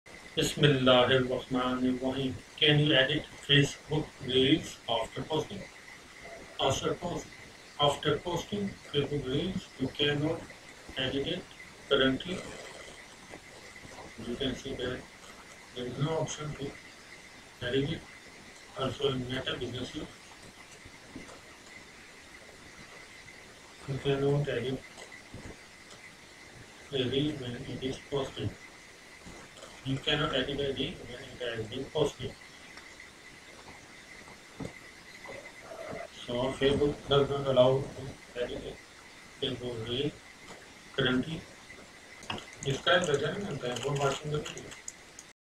How To Edit Facebook Reels After Posting (2023)
You May Also Check This Article "How to use creative tools in Facebook Reels"
Can You Edit Reels On Facebook After Sharing?
You can’t edit facebook reels (including video details i.e., captions, thumbnail, etc.,) after publishing / sharing currently. You will get the message that, "It's not possible to edit a reel post on facebook" under Meta Business Suite. However, you can edit before posting or during uploading the facebook video reel.